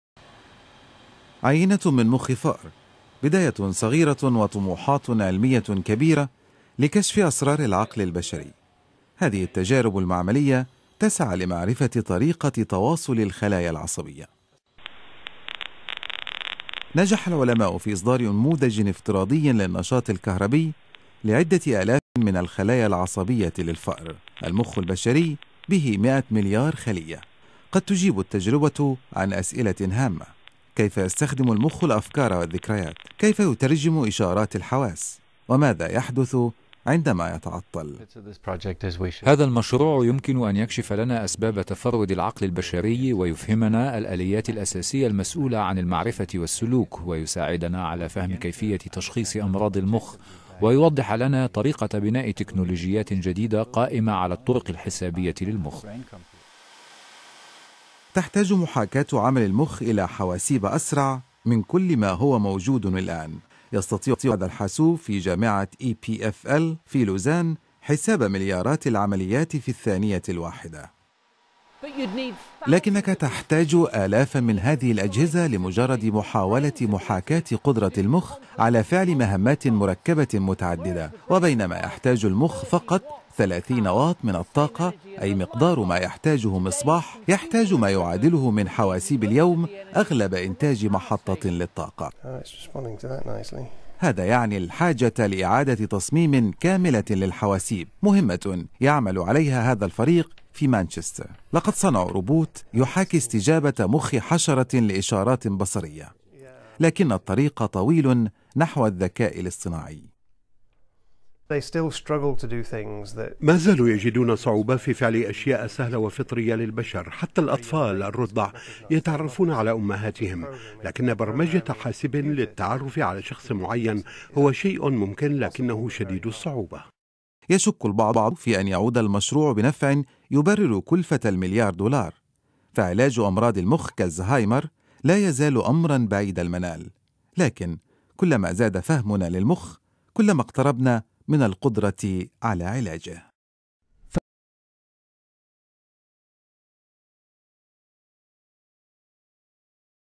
مخ فأر ومليار دولار لمحاكاة المخ البشري
تجربة لمحاكاة المخ البشري في كمبيوتر عملاق تجري في سويسرا يأمل العلماء أن تؤدي إلى فهم طريقة عمل المخ.